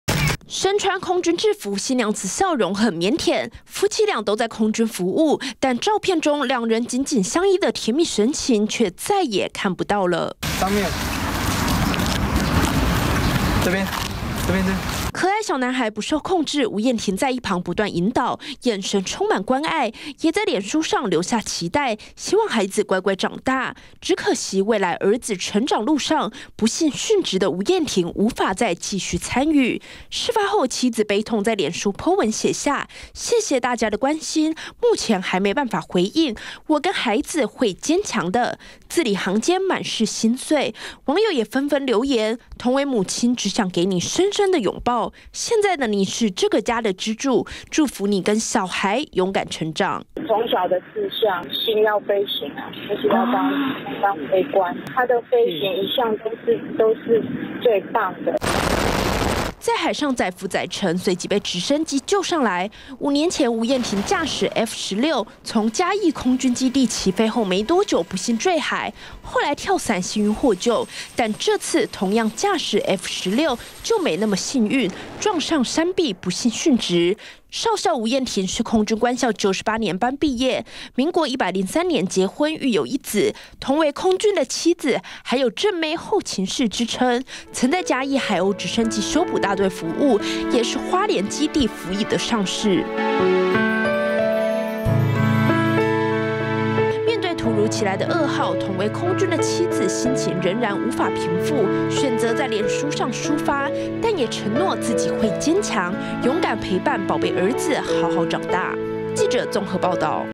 20180605中天新聞 「我跟兒子會堅強」 空軍妻PO文悼殉職夫
殉職飛官吳彥霆，其實五年前，他也曾駕駛F16戰機在嘉義墜海，當時緊急跳傘獲救，沒想到這次難逃死劫，同為空軍的妻子在臉書悲痛寫下，我跟孩子會堅強，讓人看了鼻酸。